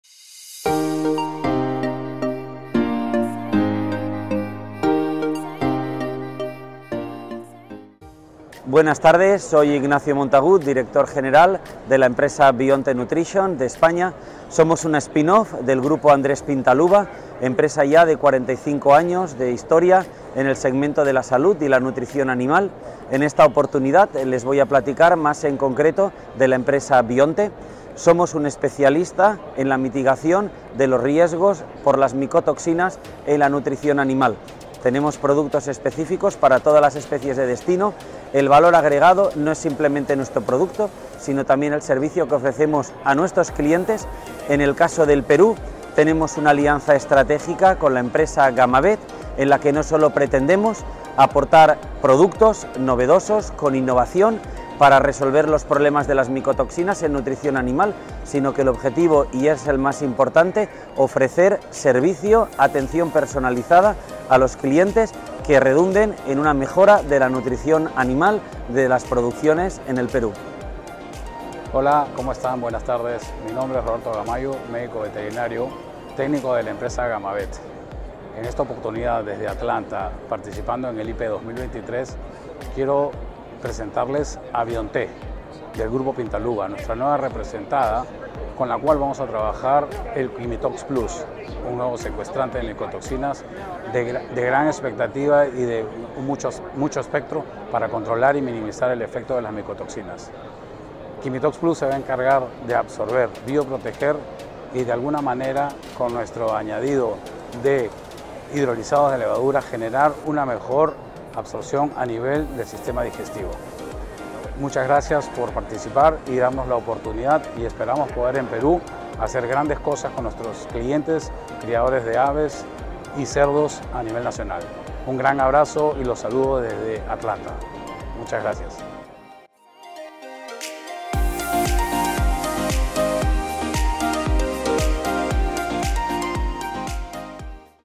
BIONTE aliado comercial de Gammavet en el IPPE 2023
Ignacio Montagud, director general de Bionte Nutrition, y Roberto Gamallo, Gerente Técnico de Gammavet, presentan la alianza comercial entre estas dos empresas líderes del sector y un nuevo producto para mitigar el efecto de las micotoxinas en la producción animal.